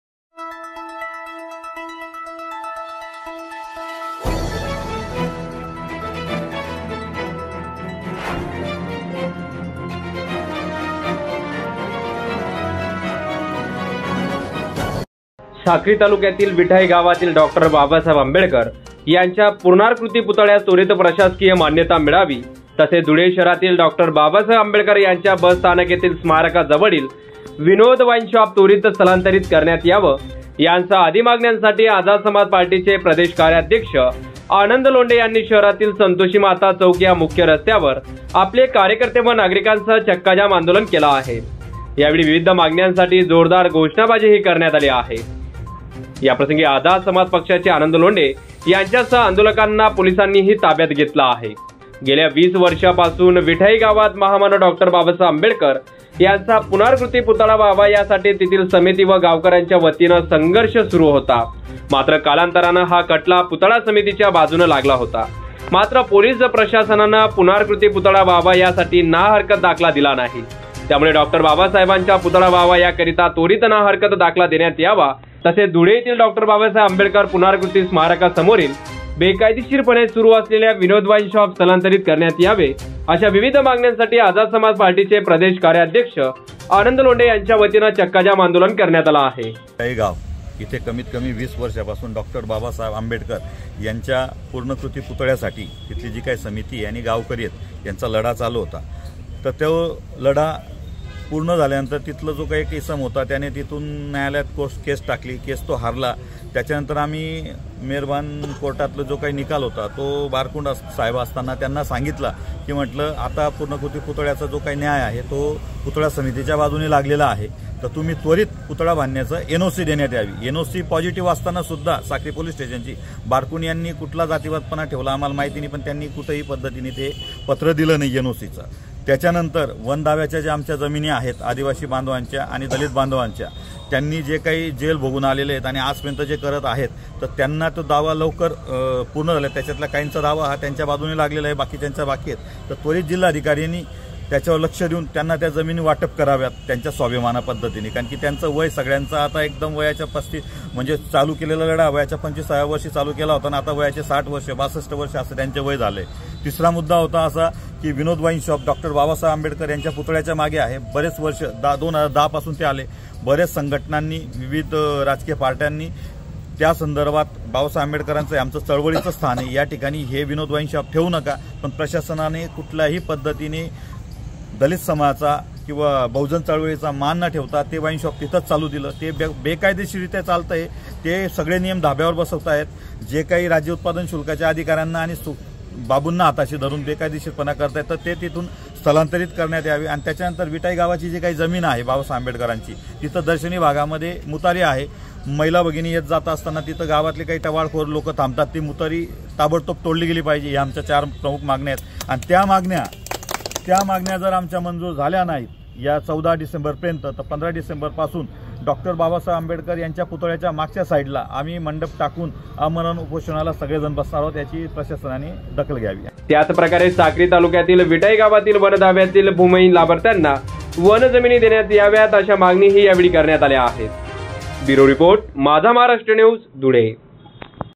आझाद समाज पार्टीचे आनंद लोंढे यांच्या नेतृत्वाखाली चक्काजाम आंदोलन....
साक्री तालुक्यातील विटाई गावातील डॉ. बाबासाहेब आंबेडकर यांच्या पुर्णाकृती पुतळ्यास त्वरीत प्रशासकीय मान्यता मिळावी. तसेच धुळे शहरातील डॉ. बाबासाहेब आंबेडकर यांच्या बस स्थानक येथील स्मारका जवळील विनोद वाईन्स शॉप त्वरीत स्थलांतरीत करण्यात यावे.यांसह आधी मागण्यासाठी आझाद समाज पार्टीचे आनंद लोंढे यांनी शहरातील संतोषी माता चौक या मुख्य रस्त्यावर आपले कार्यकर्ते व नागरिकांसह चक्काजाम आंदोलन केल आहे. यावेळी विविध मागण्यासाठी जोरदार घोषणाबाजी करण्यात आली आहे.